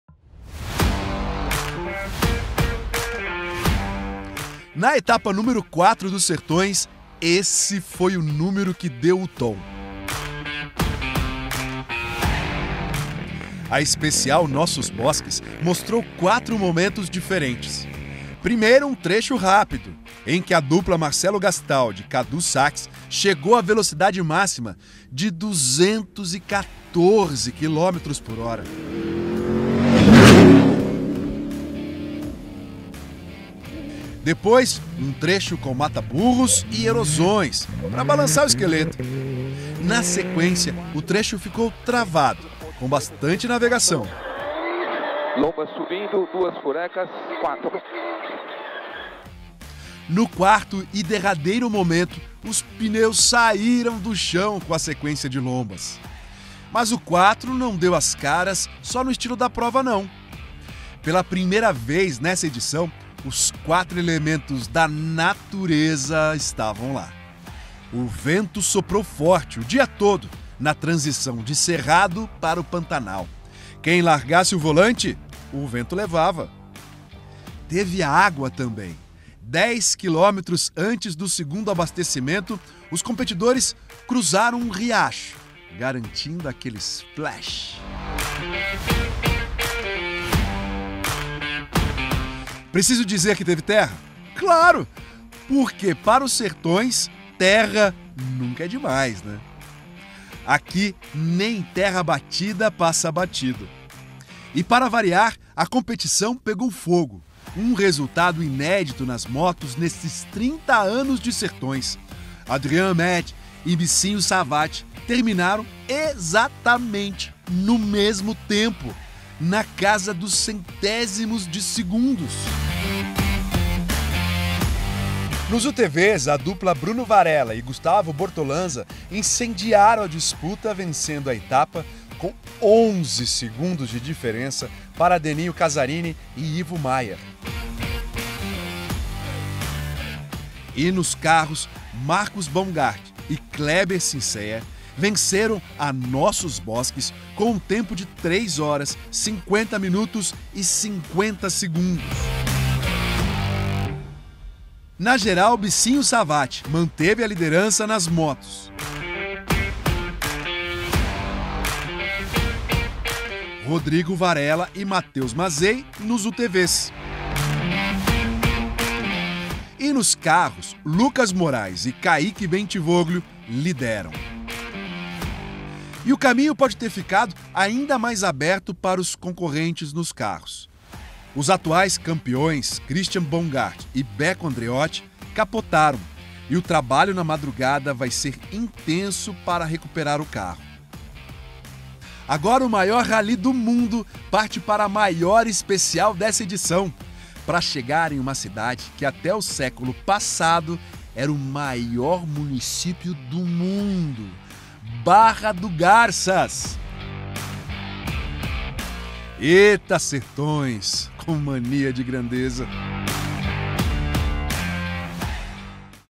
ETAPA 4 | NOSSOS BOSQUES - RESUMO - SERTÕES BRB 2022
Confira os melhores momentos da quarta etapa da edição de 30 anos do Maior Rally do mundo, realizada entre Campo Grande e Costa Rica, (MS). Com o nome de Nossos Bosques, tivemos muita ação e belíssimas paisagens nesta especial do Sertões.

Utilize o cupom